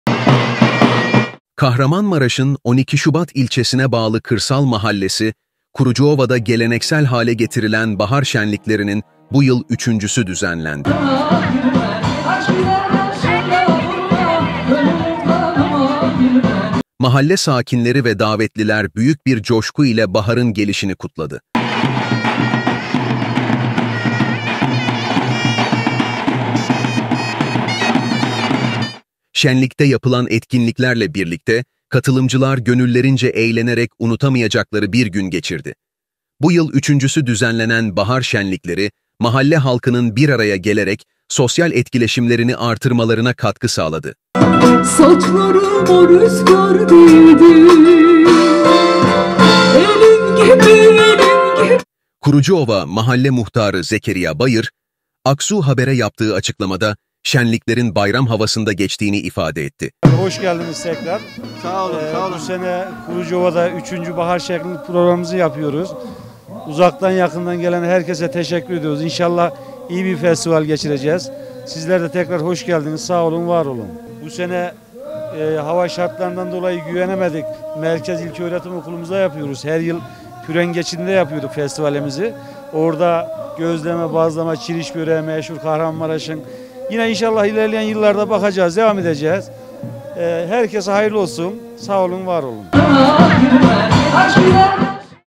Kurucuova'da Geleneksel Hale Getirilen Bahar Şenliği Düzenlendi
Kahramanmaraş'ın Onikişubat ilçesine bağlı kırsal mahallesi Kurucuova'da geleneksel hale getirilen bahar şenliklerinin bu yıl 3. düzenlendi.
Mahalle sakinleri ve davetliler büyük bir coşku ile baharın gelişini kutladı.
Şenlikte yapılan etkinliklerle birlikte katılımcılar gönüllerince eğlenerek unutamayacakları bir gün geçirdi.
Bu yıl üçüncüsü düzenlenen bahar şenlikleri mahalle halkının bir araya gelerek sosyal etkileşimlerini artırmalarına katkı sağladı.
Kurucuova Mahalle Muhtarı Zekeriya Bayır, aksu habere yaptığı açıklamada şenliklerin bayram havasında geçtiğini ifade etti.